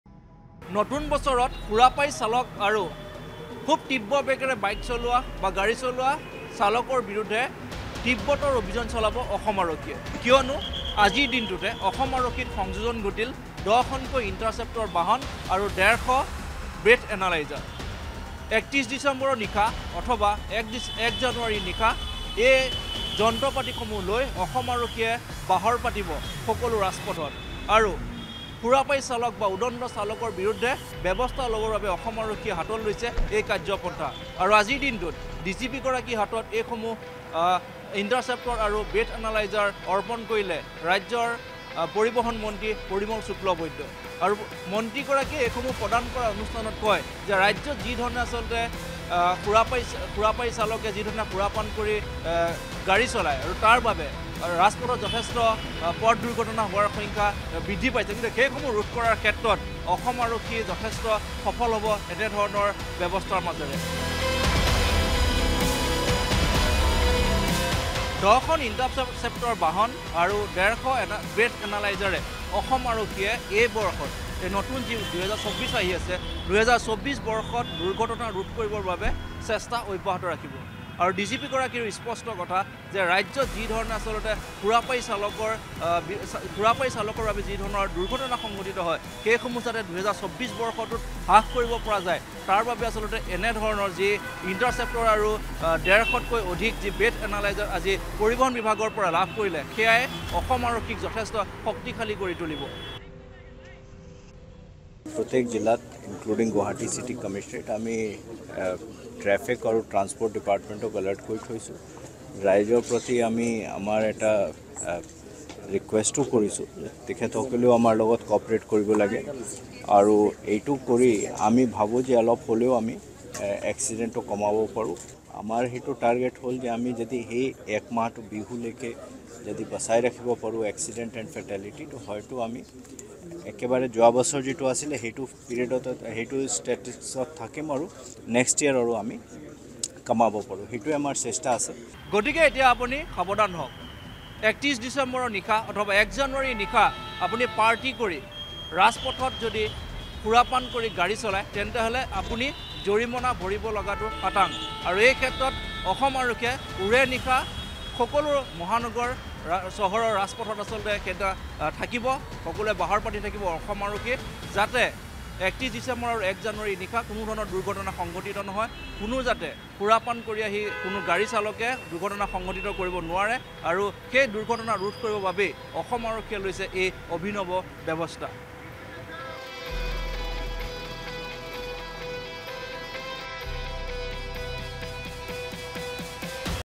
31st December Guidelines | নতুন বছৰক লৈ অধিক তৎপৰ অসম আৰক্ষী সাৱধান সুৰাপায়ী! N18V
31st December Guidelines | নতুন বছৰক লৈ অধিক তৎপৰ অসম আৰক্ষী সাৱধান সুৰাপায়ী!  N18V

লগতে চাওঁক দিনটোৰ বহু গুৰুত্বপূৰ্ণ খবৰ |